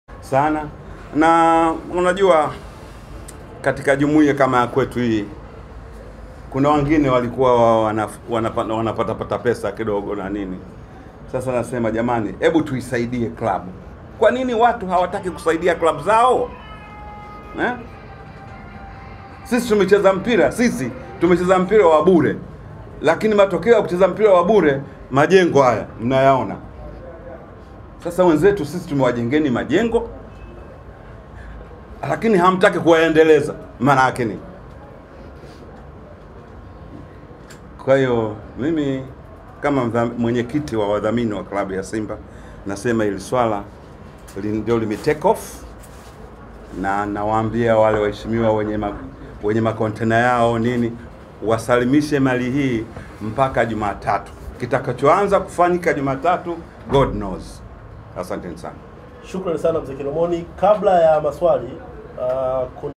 CLUB YA SIMBA NAKO KIMENUKA.WAUZA
Akizungumza na wanahabari katika ofisi hizo za Simba mwenyekiti wa wadhamini club ya Simba ndg.Hamis Ali Kilomon,kwa niaba ya club hiyo ametoa siku tano yaani kuanzia tarehe ya Sept.04/2013 kufikia  Sept 09/2013 kwa mtu yeyote anayesambaza,kuuza au kusambaza bidhaa yoyote bandia  yenye kutumia nembo  ya club ya Simba ajisalimishe kabla ya siku tano zilizobakia kufika vinginevyo hatua kali zitzchukuliwa kwa yeyote atakayebainika na kosa hilo.